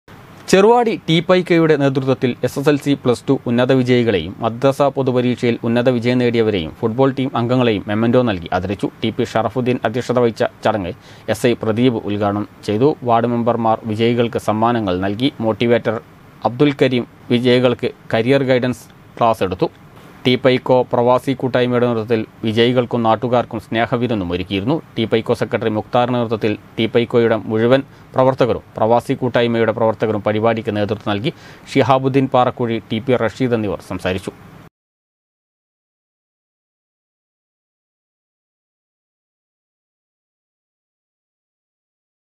ചെറുവാടി ടി. പൈക്കോയുടെ നേതൃത്വത്തിൽഎസ് എസ് എൽ സി പ്ലസ് ടു ഉന്നത വിജയികളെയും
ചെറുവാടി ടി. പൈക്കോയുടെ നേതൃത്വത്തിൽ
എസ് എസ് എൽ സി  പ്ലസ് ടു ഉന്നത വിജയികളെയും മദ്രസ പൊതുപരീക്ഷയിൽ ഉന്നത വിജയികളെയും ഫുട്ബോൾ ടീം അംഗങ്ങളെയും മൊമന്റോ നൽകി ആദരിച്ചു